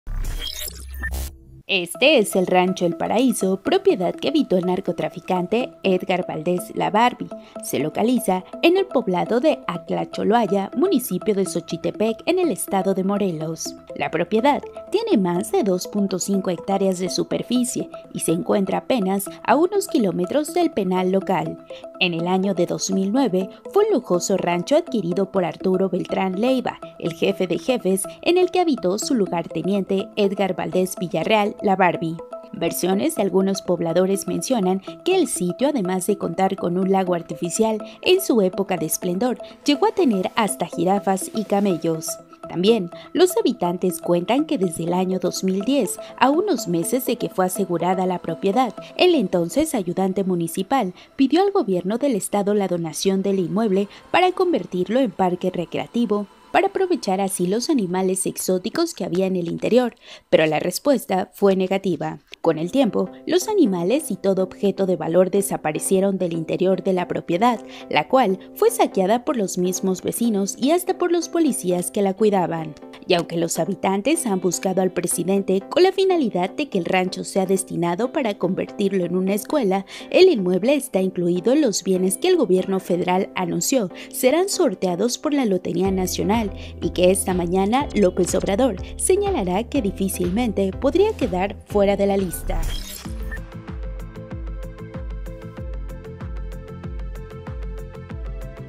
Así es el rancho que habitó el narcotraficante Édgar Valdez, “La Barbie”, y que será rifado
El inmueble, que fue habitado por el narcotraficante Édgar Valdez, La Barbie, se encuentra en Atlacholoaya, Morelos, y ahora será sorteado por la Lotería Nacional